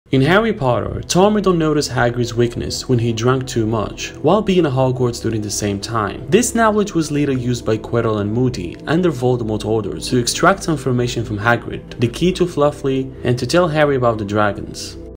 Did you notice this Tom Riddle/Hagrid connection in Harry Potter?
In Harry Potter, Tom Riddle noticed Hagrid's weakness when he drank too much, while being at Hogwarts during the same time. This knowledge was later used by Quirrell and Moody, under Voldemort's orders, to extract information from Hagrid: the key to Fluffy and to tell Harry about the dragons.